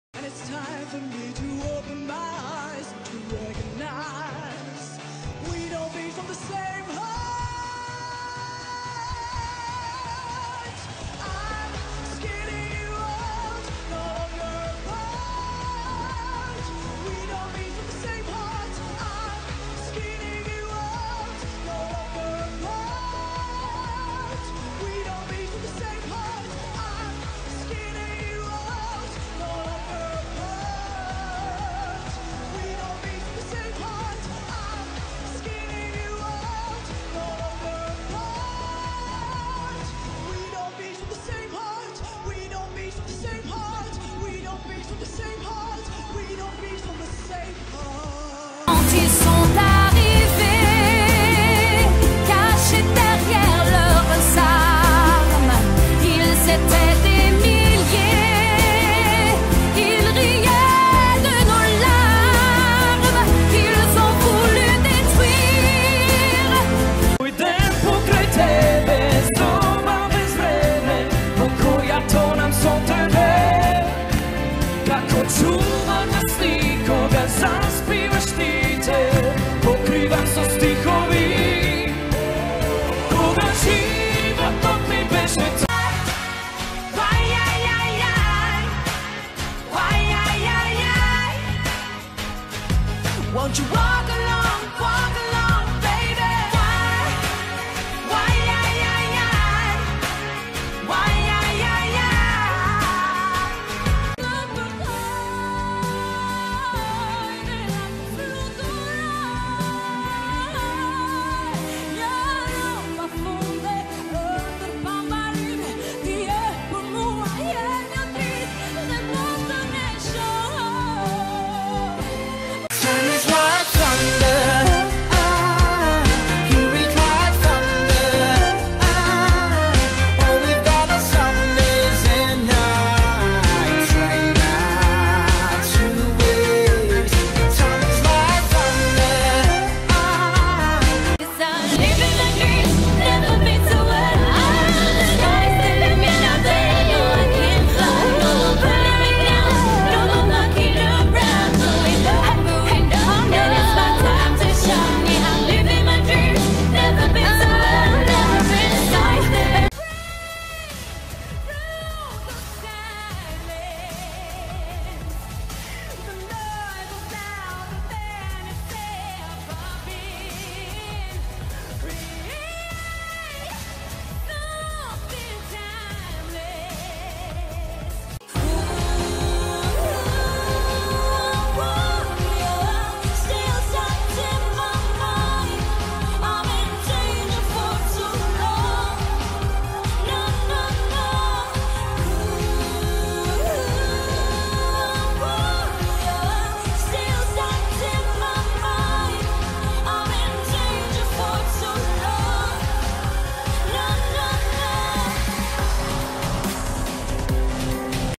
Eurovision 2015 My top 8 So far (01/02/15)
Hi everybody ! This is my personnal Top 8 of the ESC 2015 ! I love all these songs so don't blame me for my taste. Sorry for my bad english, Hayana :3.